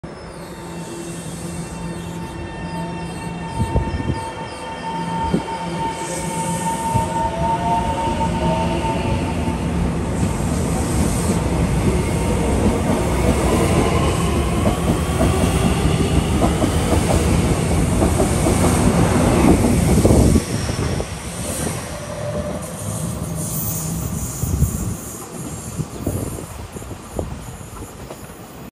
2203次 EMU700區間車 楊梅站南下開車
2203次 EMU700區間車 經山線往嘉義